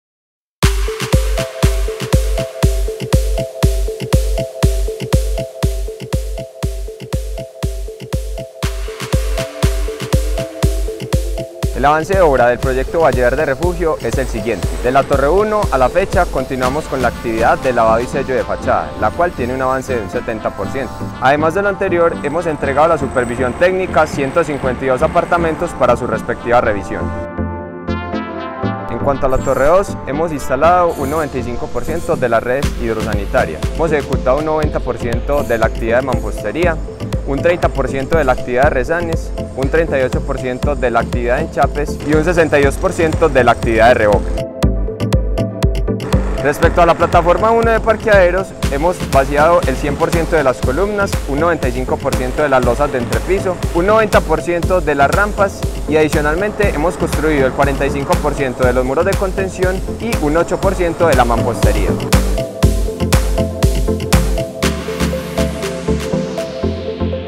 Avance de Obra Valle Verde Refugio Julio 2023
🏡 Valle Verde Refugio en Bello, Antioquia, avanza con paso firme: 🌱

Torre 1:

🏢 Lavado de Fachada: 70%

🔑 152 apartamentos entregados

Torre 2:

🚿 Redes hidrosanitarias: 95%

🧱 Mampostería: 90%

🔨 Resanes: 30%

🔧 Enchapes: 38%

✅ Revoque: 62%

Parqueaderos:

🚗 Columnas: 100%

🛑 Losas de entrepiso: 95%

⬆️ Rampas: 90%

🧱 Muros de contención: 45%

🔧 Mampostería: 8%